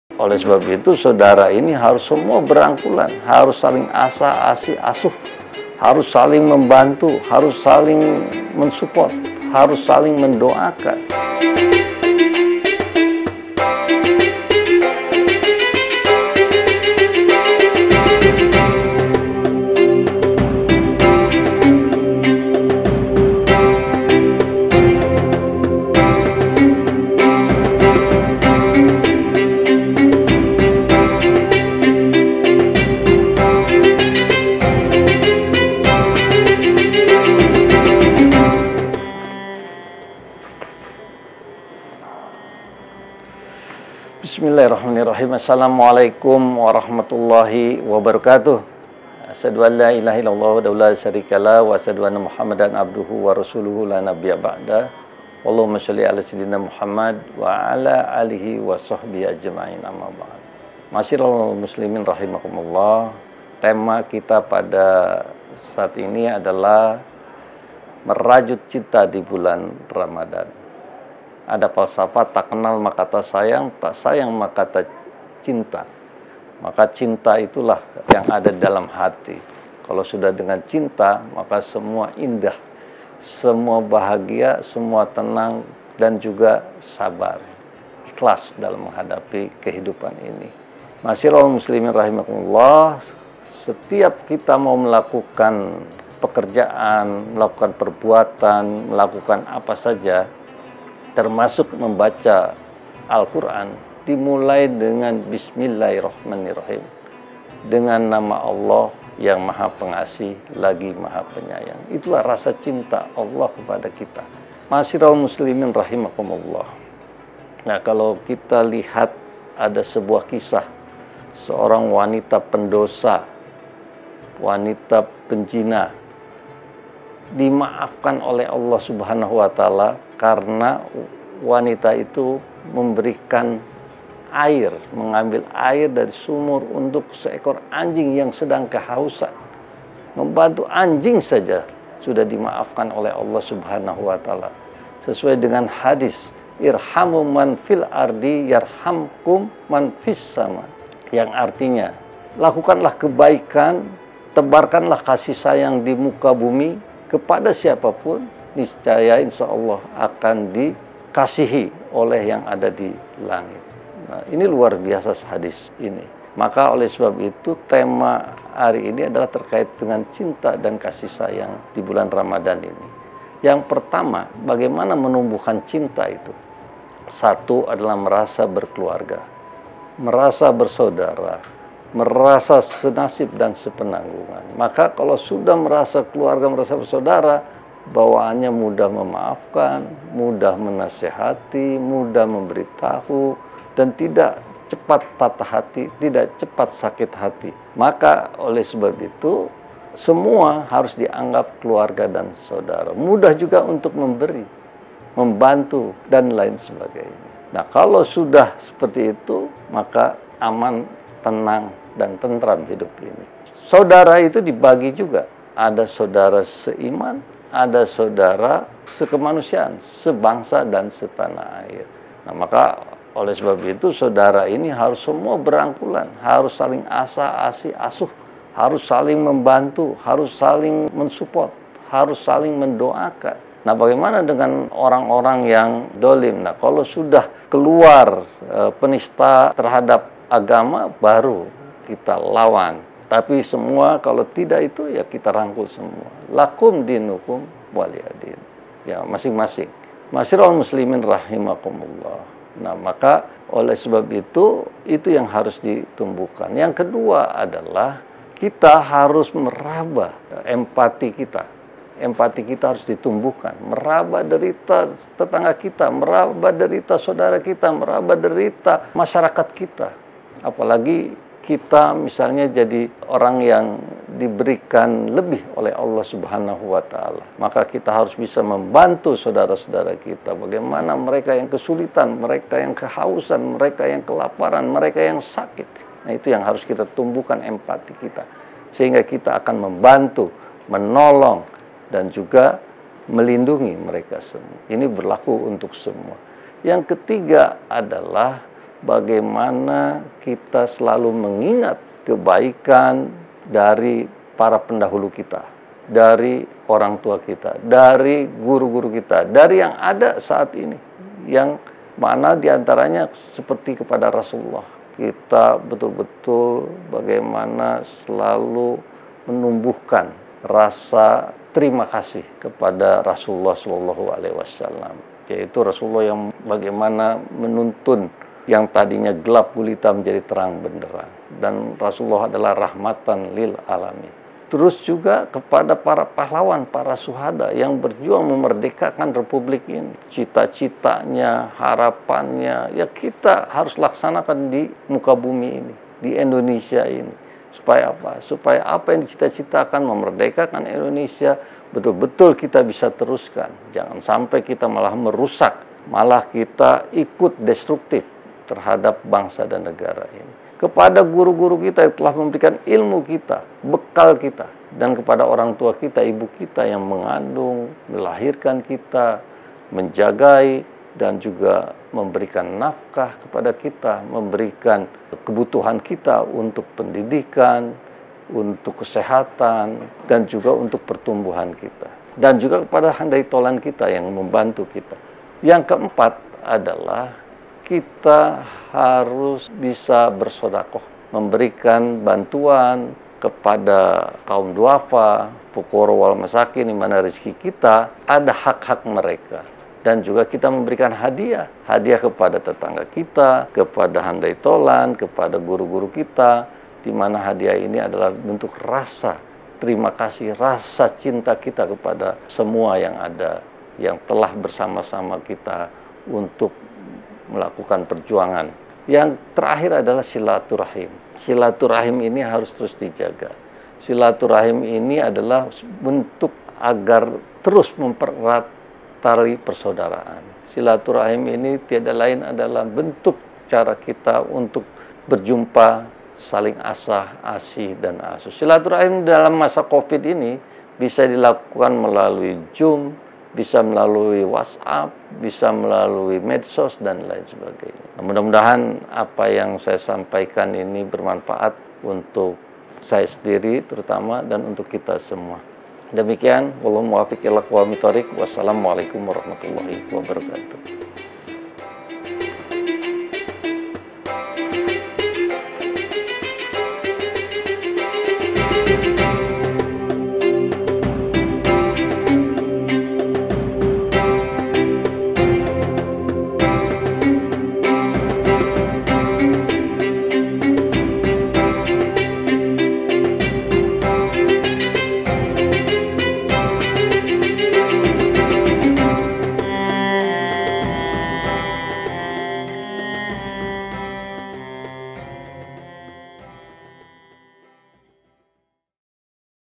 MERAJUT CINTA DI BULAN RAMADAN
Saksikan Kurma DPR dengan tema "Merajut Cinta Di Bulan Ramadan" bersama Wakil Ketua BURT DPR RI Achmad Dimyati Natakusumah pada tayangan berikut ini. BulanPuasa​​​ Ramadan​​​ RamadanKareem​​​ RamadanMubarak​​​ KurmaDPR​​​